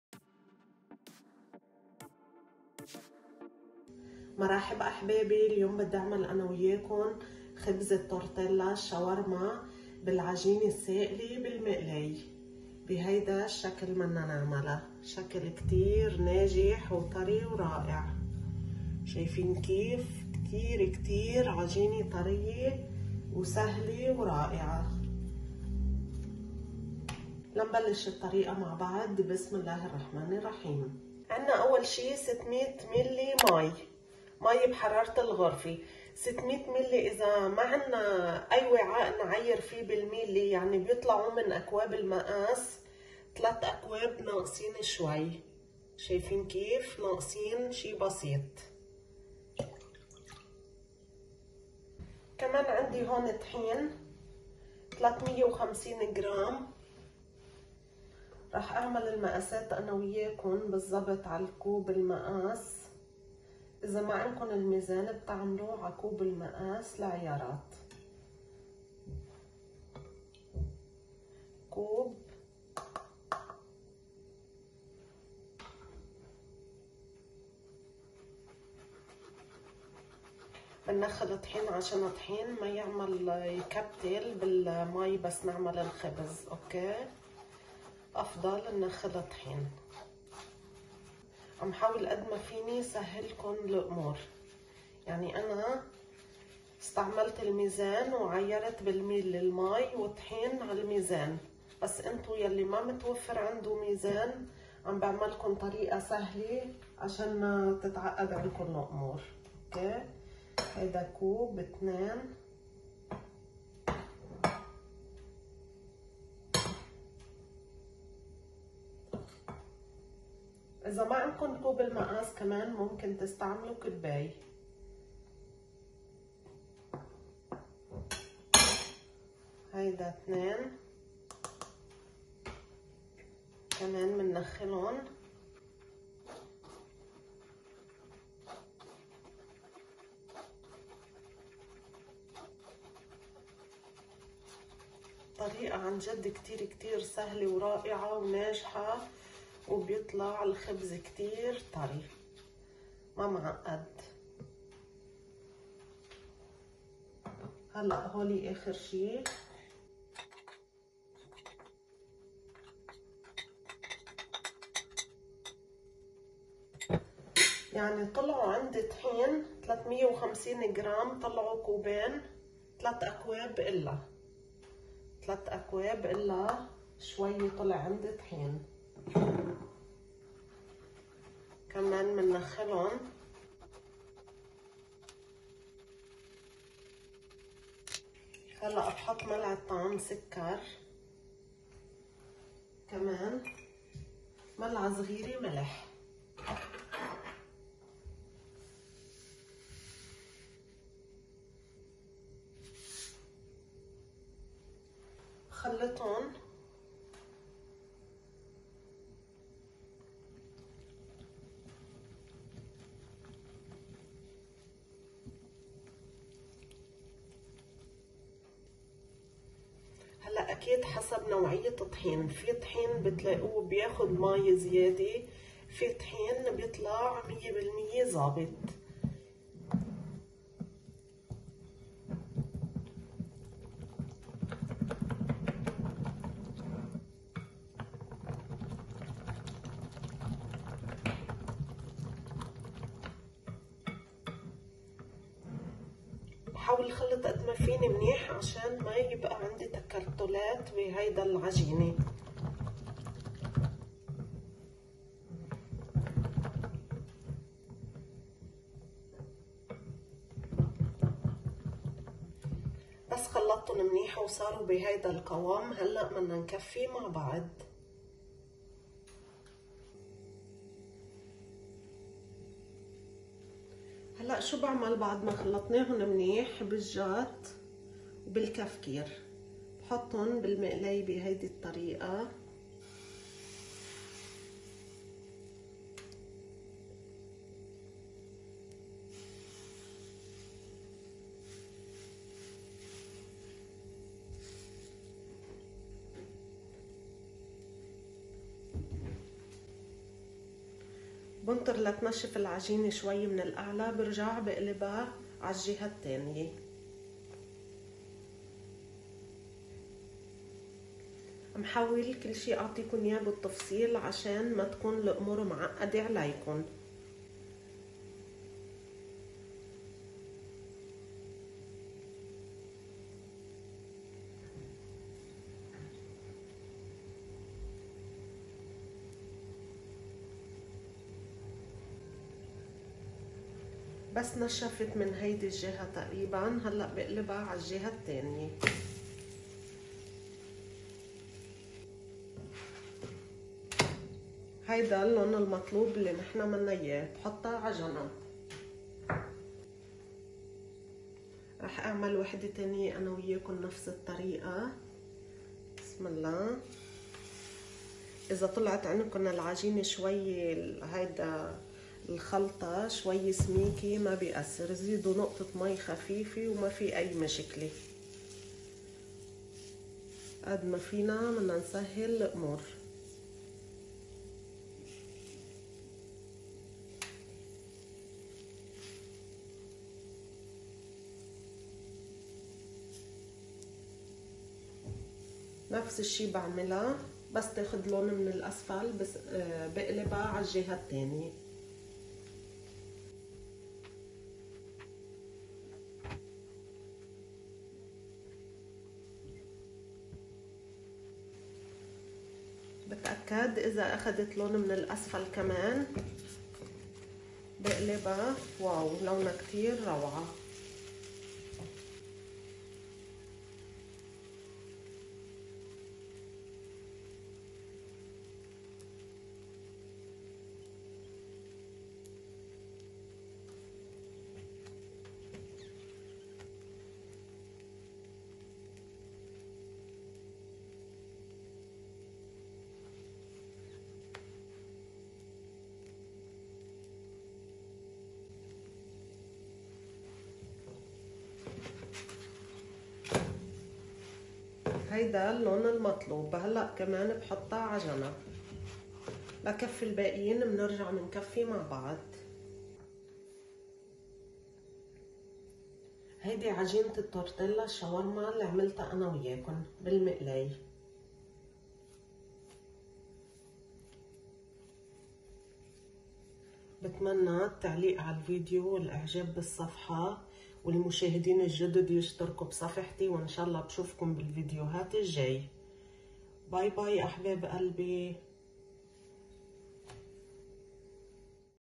خبز التورتيلا والشاورما بعجينة سائلة بدون خميرة وبدون عجن
المقادير
٦٠٠ مل ماي
٤٠٠ غ طحين
١ ملعقة كبيرة سكر
١ ملعقة صغيرة ملح

Ingredients
600 ml of water
400 g of flour
1 tablespoon of sugar
1 teaspoon of salt